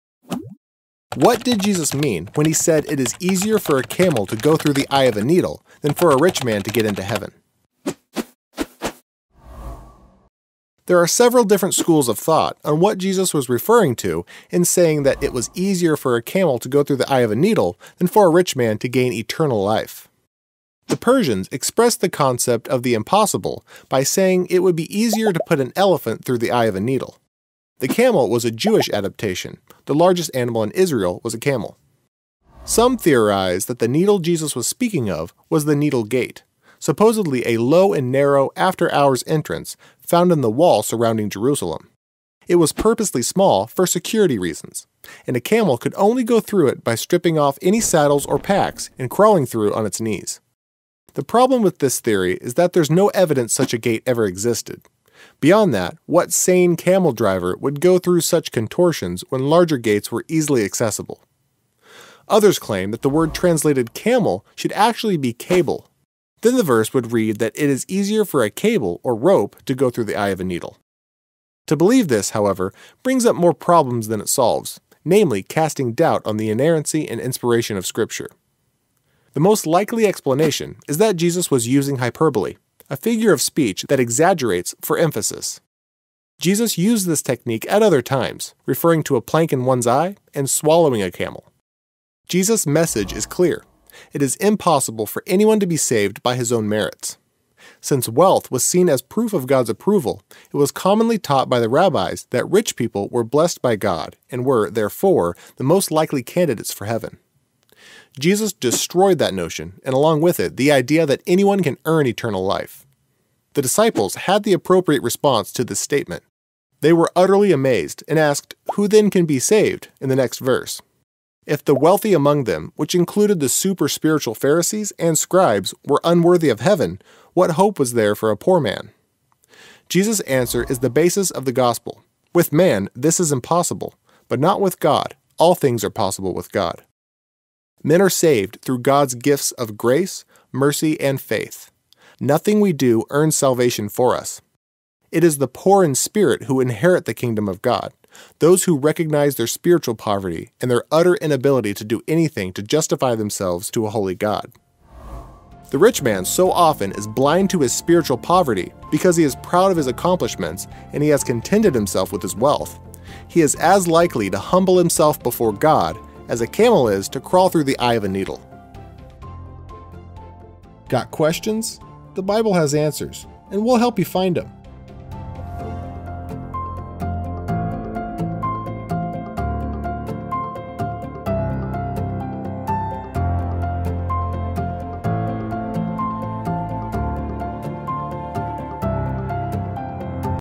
What did Jesus mean when He said it is easier for a camel to go through the eye of a needle?
What did Jesus mean when He said it is easier for a camel to go through the eye of a needle than for a rich man to get into heaven? Was Jesus referring to a literal camel and a literal needle, or to a gate that was known as the Needle’s Eye?